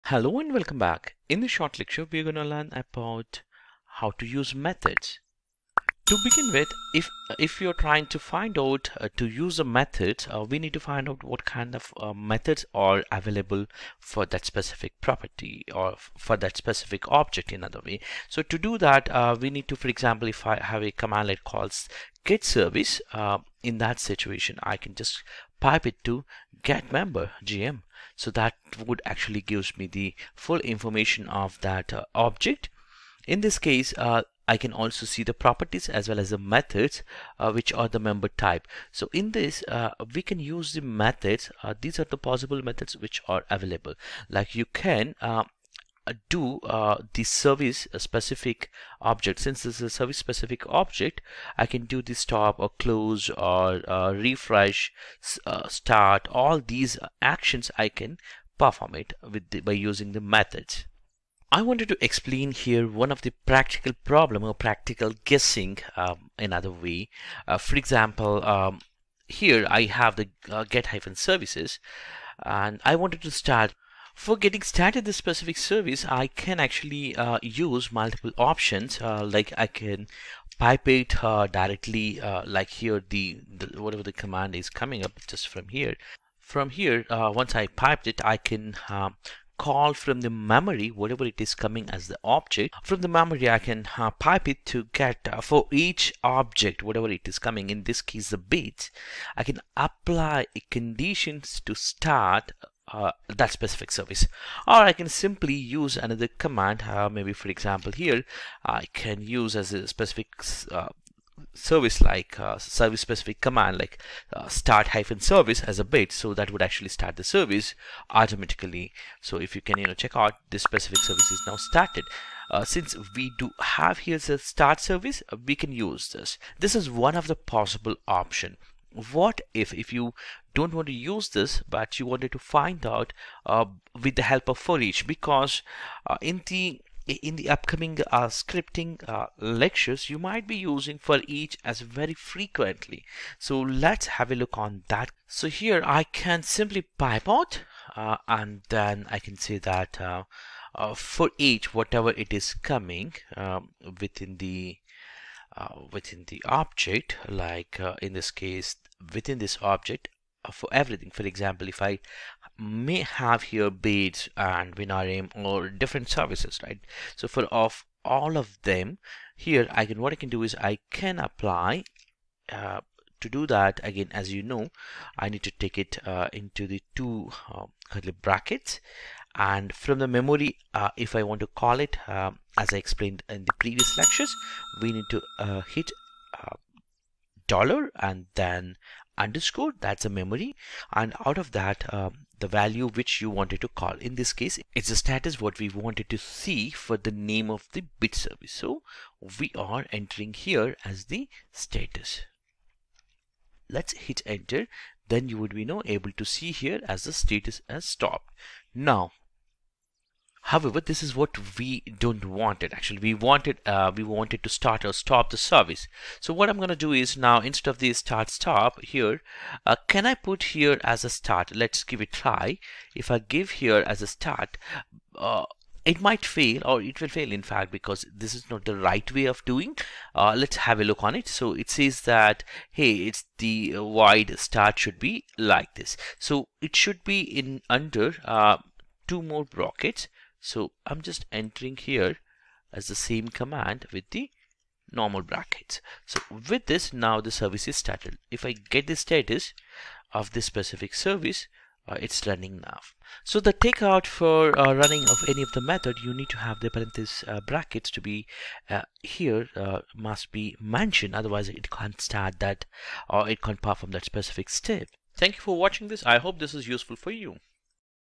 Learn PowerShell Object Methods how to use
This lecture covers below topics
Using PowerShell Object Methods
How to use methods in PowerShell
Powershell method Example
Powershell method vs Function
Powershell method Parameters
Powershell methods basics